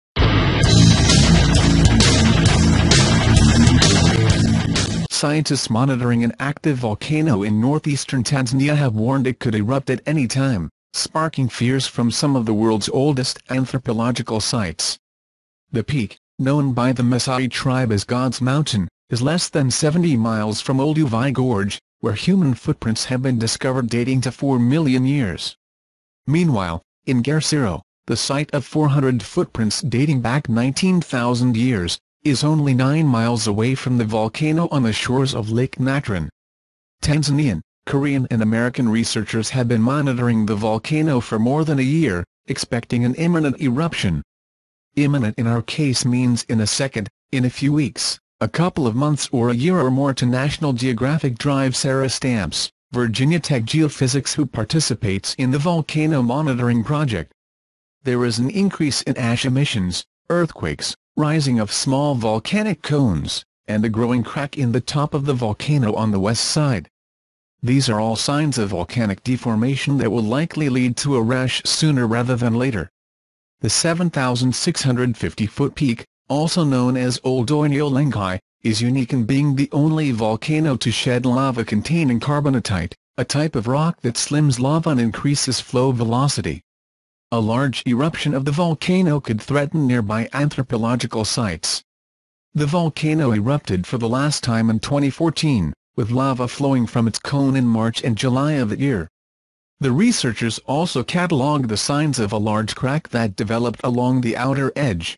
Mountain of God The eruption of the volcano: it is imminent warn the observers.
Scientists monitoring an active volcano in northeastern Tanzania have warned it could erupt at any time, sparking fears from some of the world's oldest anthropological sites. The peak, known by the Maasai tribe as God's Mountain, is less than 70 miles from Olduvai Gorge, where human footprints have been discovered dating to 4 million years. Meanwhile, Engare Sero, the site of 400 footprints dating back 19,000 years, is only nine miles away from the volcano on the shores of Lake Natron.

Tanzanian, Korean and American researchers have been monitoring the volcano for more than a year, expecting an imminent eruption. Imminent in our case means in a second, in a few weeks, a couple of months or a year or more to National Geographic Dr. Sarah Stamps, Virginia Tech geophysics who participates in the volcano monitoring project. There is an increase in ash emissions, earthquakes, rising of small volcanic cones, and a growing crack in the top of the volcano on the west side. These are all signs of volcanic deformation that will likely lead to a rash sooner rather than later.

The 7,650-foot peak, also known as Ol Doinyo Lengai, is unique in being the only volcano to shed lava containing carbonatite, a type of rock that slims lava and increases flow velocity. A large eruption of the volcano could threaten nearby anthropological sites. The volcano erupted for the last time in 2014, with lava flowing from its cone in March and July of that year. The researchers also cataloged the signs of a large crack that developed along the outer edge.